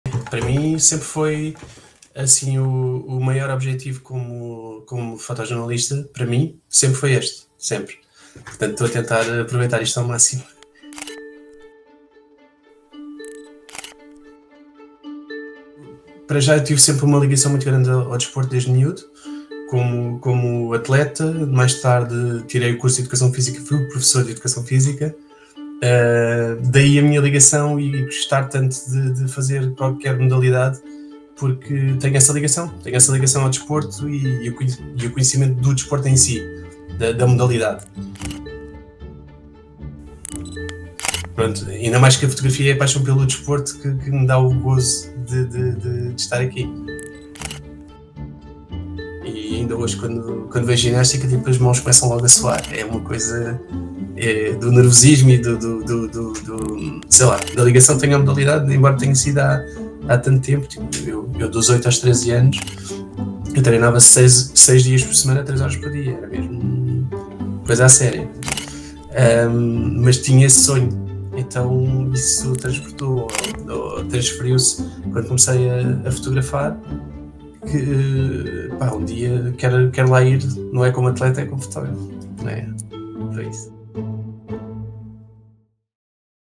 Sonhava com os Olímpicos quando era ginasta, chegou lá como fotojornalista
José Sena Goulão conta que "o maior objetivo como fotojornalista" sempre foi cobrir os Jogos Olímpicos